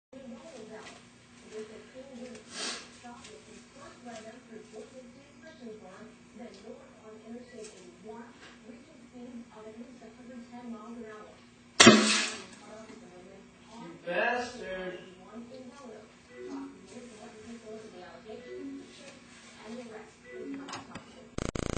Beer Popping
Same philosophy as tapping a beer bottle on the top to make it foam over, but done out the other end.